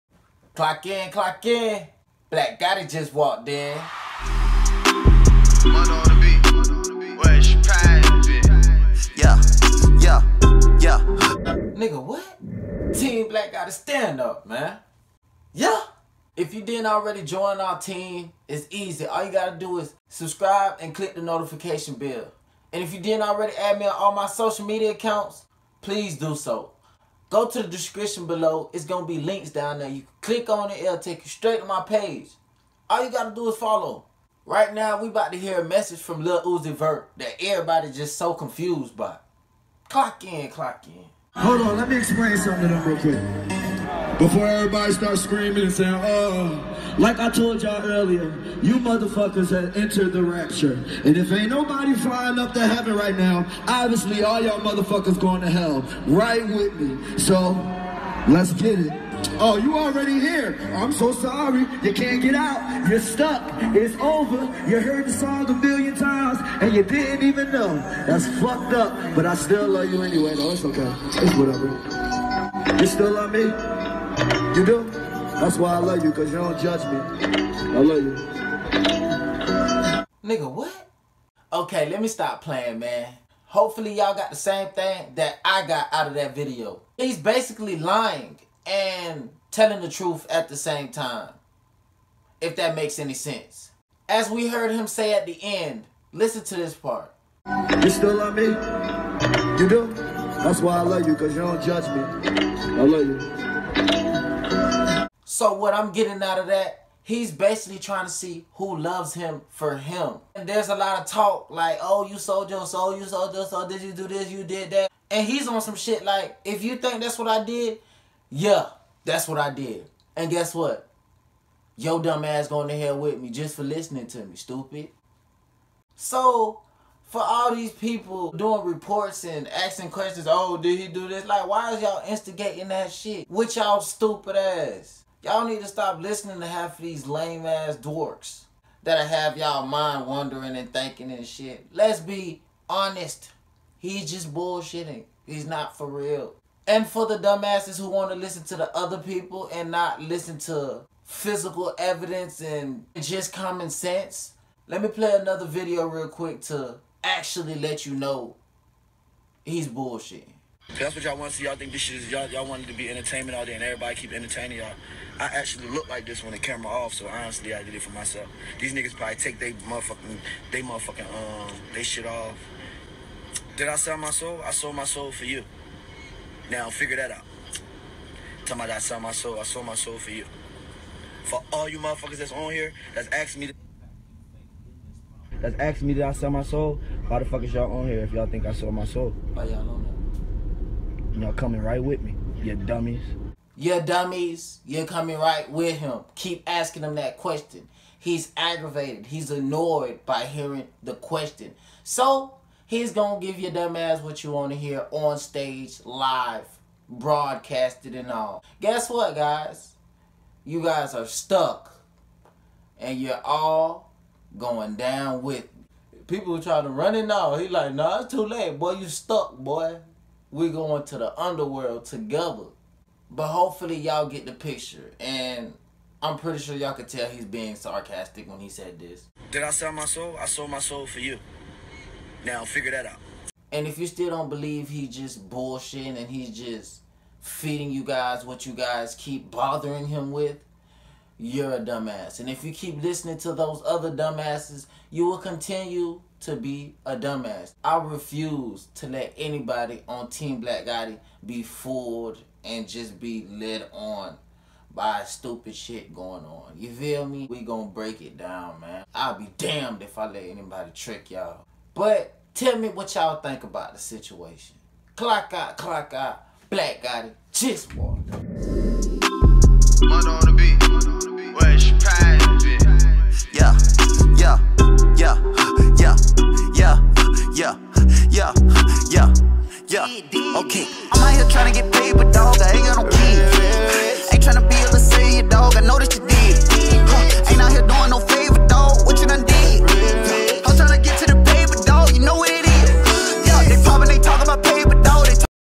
Truth About Lil Uzi Vert Telling Fans They Are Going To Hell With Him
Can’t believe he said this after